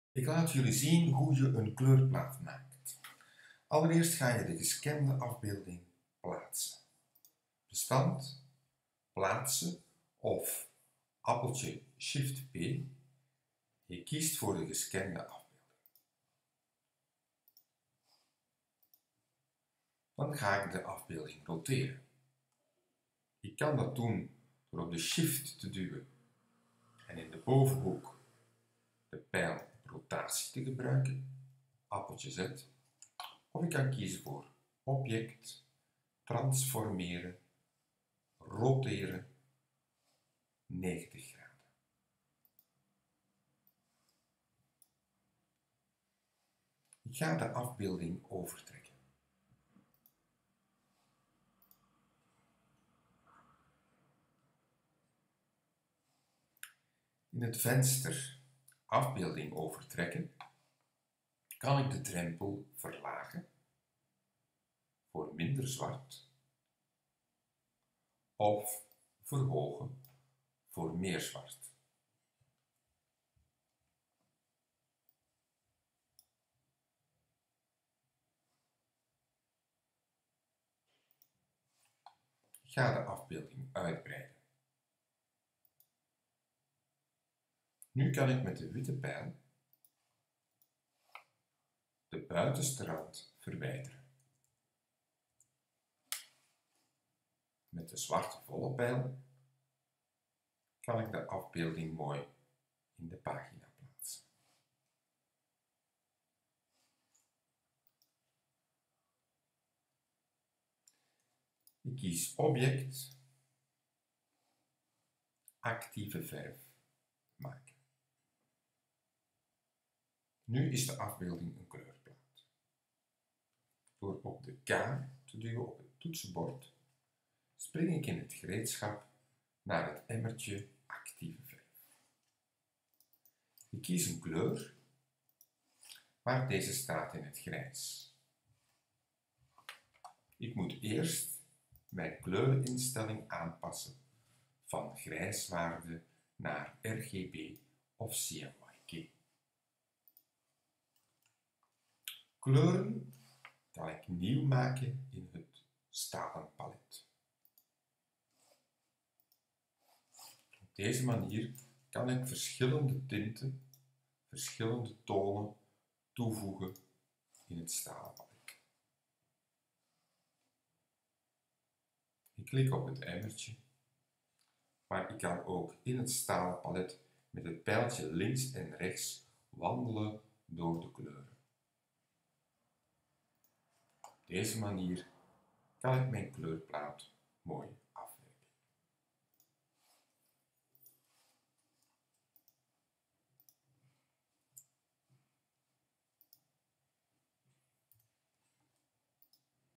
Kleurplaat maken in illustrator
Hoe maak ik een kleurplaat (actieve verf) in illustrator?